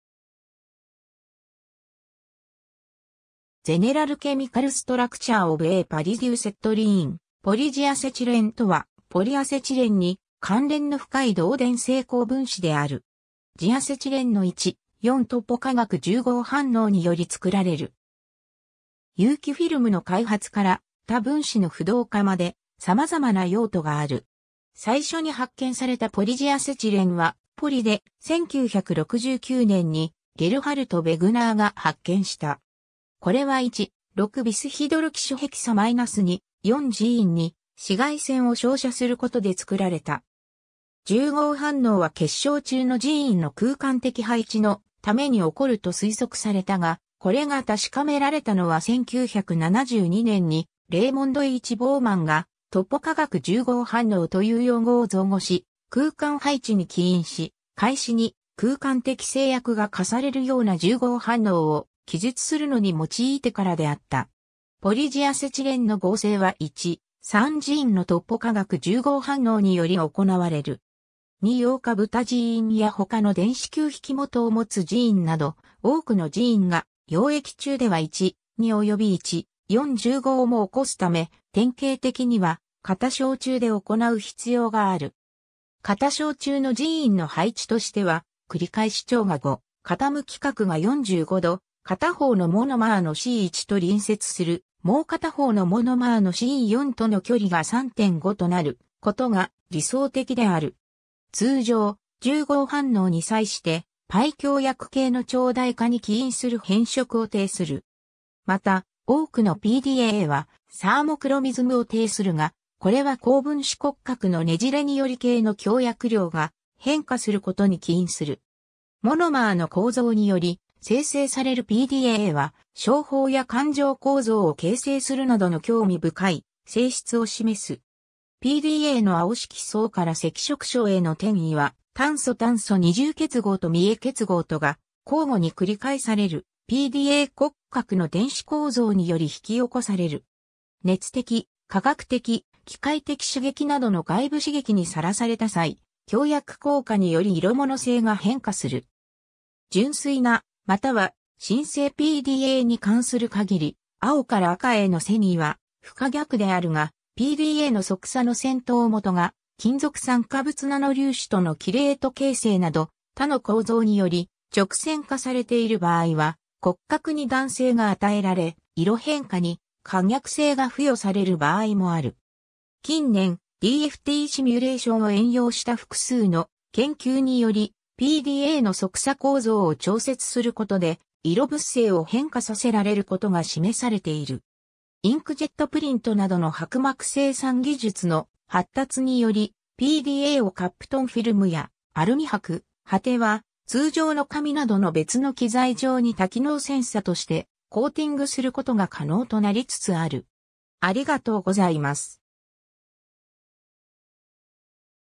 ポリジアセチレン
General chemical structure of a polydiacetylene ポリジアセチレン (英: Polydiacetylene, PDA) とは、ポリアセチレンに関連の深い導電性高分子である。
ジアセチレン（ブタジイン）の 1,4 トポ化学重合反応により作られる。
有機フィルムの開発から他分子の不動化まで、様々な用途がある。
 最初に発見されたポリジアセチレンはポリ(1,6-ビスヒドロキシヘキサ-2,4-ジアセチレン) で、1969年にゲルハルト・ヴェグナーが発見した。
これは1,6-ビスヒドロキシヘキサ-2,4-ジインに紫外線を照射することで作られた 。
重合反応は結晶中のジインの空間的配置のために起こると推測されたが、これが確かめられたのは1972年にレイモンド・H・ボーマンが「トポ化学重合反応」という用語を造語し、空間配置に起因し、開始に空間的制約が課されるような重合反応を記述するのに用いてからであった。
 ポリジアセチレンの合成は1,3-ジインのトポ化学重合反応により行われる。
2ヨウ化ブタジインや他の電子求引基を持つジインなど、多くのジインが溶液中では1,2および1,4重合も起こすため、典型的には固相中で行う必要がある。
固相中のジインの配置としては、繰り返し長が 5Å、傾き角が 45°、片方のモノマーのC1と隣接するもう片方のモノマーの C4 との距離が 3.5Å となることが理想的である。
 通常、重合反応に際してπ共役系の長大化に起因する変色を呈する。
また、多くのPDAはサーモクロミズムを呈するが、これは高分子骨格のねじれにより系の共役量が変化することに起因する。
モノマーの構造により、生成されるPDAは小胞や管状構造を形成するなどの興味深い性質を示す。
 PDAの青色相から赤色相への転移は、炭素-炭素二重結合と三重結合とが交互に繰り返される、PDA骨格の電子構造により引き起こされる。
熱的、化学的、機械的刺激などの外部刺激に晒された際、共役効果により色物性が変化する。
純粋な、または「真性」 PDAに関する限り、青から赤への遷移は不可逆であるが、PDAの側鎖の先頭基が、金属酸化物ナノ粒子とのキレート形成など他の構造により直線化されている場合は、骨格に弾性が与えられ、色変化に可逆性が付与される場合もある。
近年、DFTシミュレーションを援用した複数の研究により、PDAの側鎖構造を調節することで色物性を変化させられることが示されている。
インクジェットプリントなどの薄膜生産技術の発達により、PDAをカプトンフィルムやアルミ箔、果ては通常の紙などの別の基材上に多機能センサとしてコーティングすることが可能となりつつある。